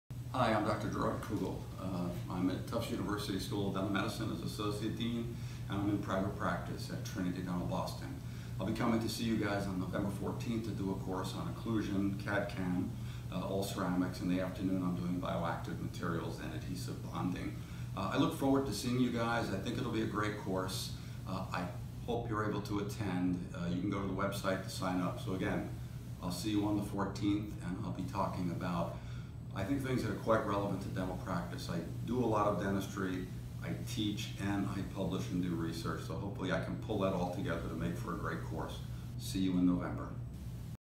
CSDA CE Package Program: Gerard Kugel, DMD (November 2018)
Gerard Kugel, DMD

"Occlusion, All Ceramic and CAD/CAM Dentistry, State of the Art" (AM session)

The choice of materials we can use for our fixed prosthetics has changed dramatically over the past 10 years. Porcelain to metal, along with gold, are still the benchmarks by which we judge all other materials. However, our patients are pushing the esthetic envelope. We must be aware of what our options are and, more importantly, the indications and contraindications for their use. We will review the core principles of esthetic dentistry as they relate to tooth form and function. Methods for obtaining correct bite registrations, perio-esthetics, anterior guidance, and equilibration will also be reviewed. Finally, this lecture will review all-ceramic materials, including lab processed and chairside CAD/CAM crowns, digital impressioning and cement options. An emphasis will be placed on clinical applications, including indications and preparation design. Clinical data will be reviewed.

Learning Objectives:
• Learn what materials work best and in which circumstances
• Understand occlusion with all ceramic dentistry
• Review the pros & cons of digital dentistry
• Discuss long term maintenance of your esthetic cases

"Bioactive Dentistry: Prevention to Restoration" (PM session)

This course will cover everything from prevention to restoration, highlighting the new class of bioactive/biomimetic materials. We will review techniques and tools to enhance the long-term maintenance of your patient’s dentition and to help prevent the need for future restoration. Questions are arising as to the longevity and appropriateness of our current resin material systems and the new generation of bonding agents. Researchers have begun to develop advanced acid-base restorative alternatives to help with the limitations we see with our restorative and luting needs. We will discuss these new materials and how they can help us produce better, longer lasting bioactive and protective restorations, as well as improve our bonding results. You will be introduced to direct restorative techniques using bioactive
materials that will easily enhance your practice and benefit your patients.

Learning Objectives:
• Review traditional materials and why they may need an “upgrade”
• Learn what bioactive materials are and how they work
• Discover how bioactive materials can replace traditional materials
• Understand why resin bonded restorations and bonding systems are being questioned